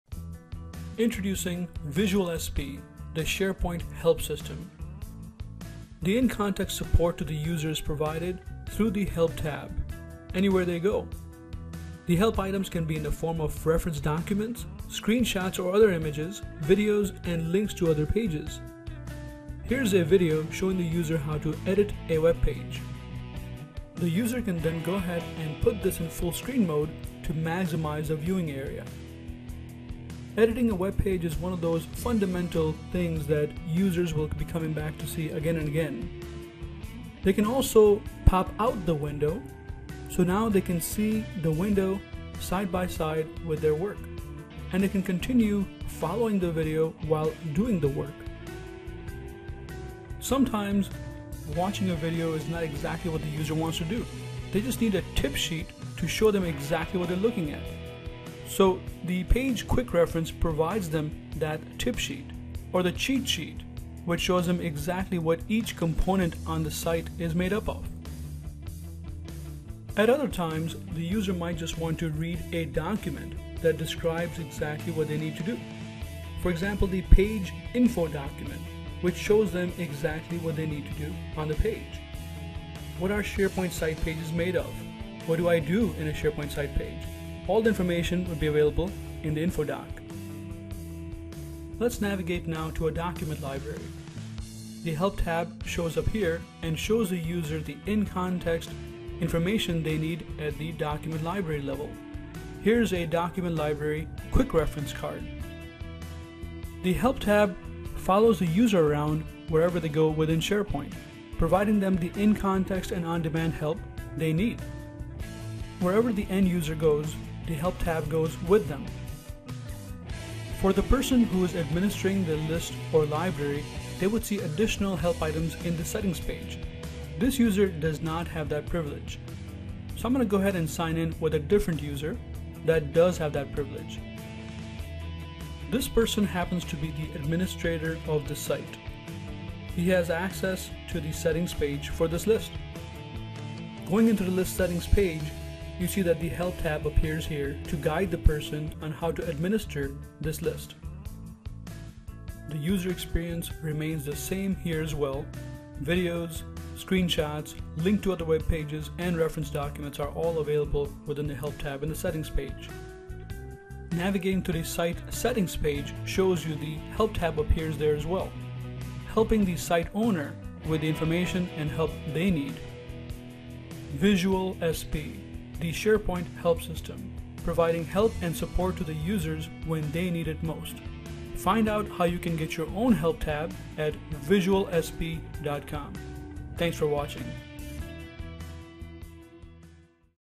VisualSP for SharePoint 2013 - In-context support for SharePoint users
Support your SharePoint users with the SharePoint Help tab! Provide help when They need it!

VisualSP provides on-demand and in-context SharePoint user support through the SharePoint Ribbon. Video tutorials, help documents, screenshots and link to web pages are available in-context to the users while working within their SharePoint environment helping them be successful with their tasks.